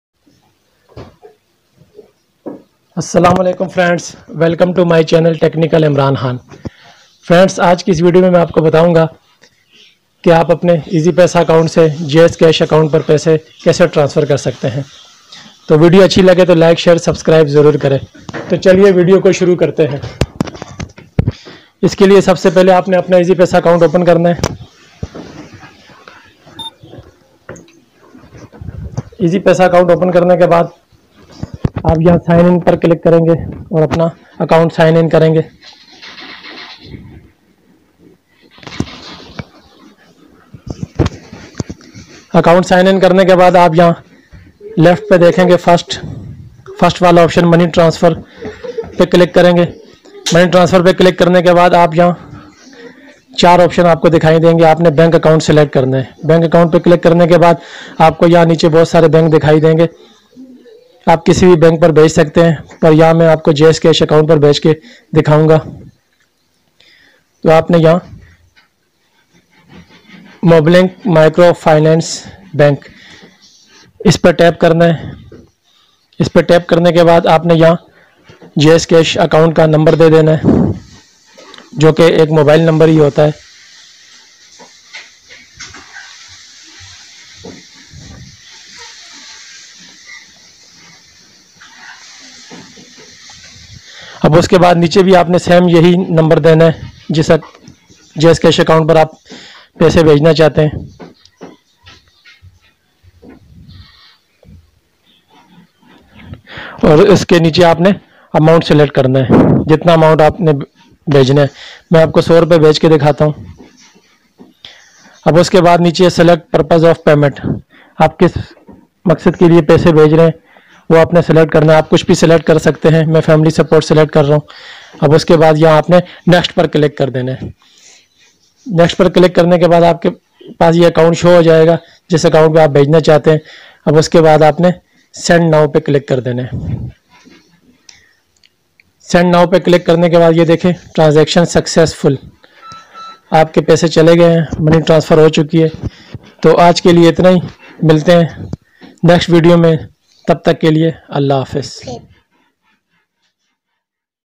How to Transfer FREE Money From Easypaisa to JazzCash Mobile account 2020 by Technical Imran khan
Assalaam o Alaikum Dosto aaj ki is video main aap ko bataya gya hay k aap asani k sath apne easypaisa account se Jazz cash account per money transfer kese ker sakte hain Dosto bhuat hi asaan tarike se is video main aap ko bataya hay jo k umeed hay aap ko samajh a jaye ga.

please
Technical Imran khan